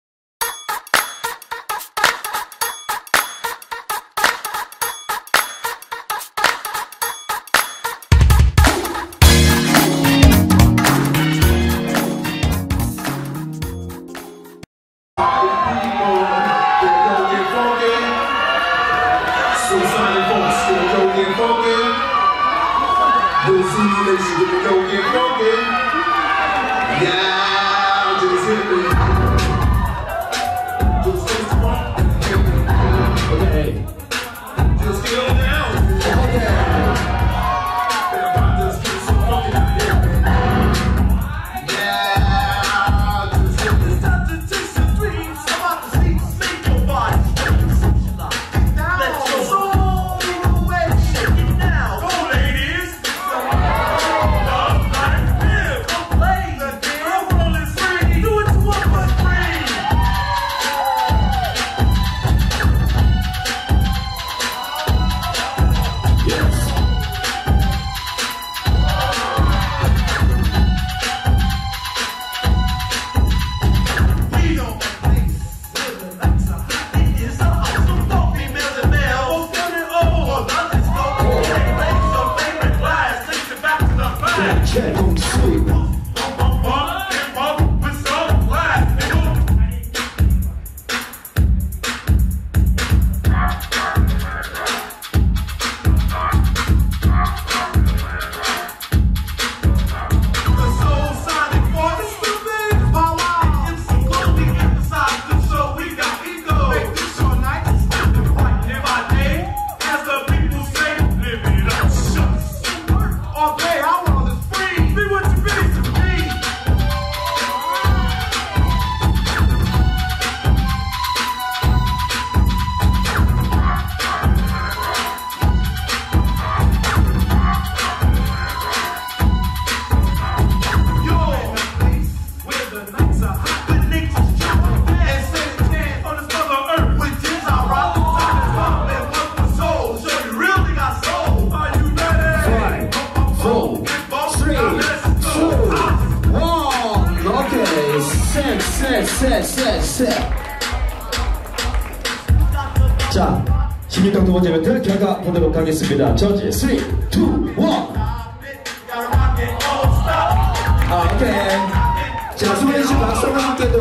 2024 DAMOIN VOL.3 16강 | 다모인 3| BEST16 |직장인 댄서 배틀| 다모인 3 올스타일 배틀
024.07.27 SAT
DAMOIN VOL.3 [다모인]
본격 뒷풀이를 위한 배틀&퍼포먼스!
JUDGE
BATTLE SIDE
:BEAST [B-BOY]
:D.B.SHINJI [LOCKING]
:AMIN [POPPING]
:ZAPPY [VOL.2우승자]
PERFORMANCE SIDE
:FUNKY-Y [MANNEQUEEN]
DJ
:ZINWON [BATTLE]
:JOOPOP [PARTY]

MC
:SHAR-PEI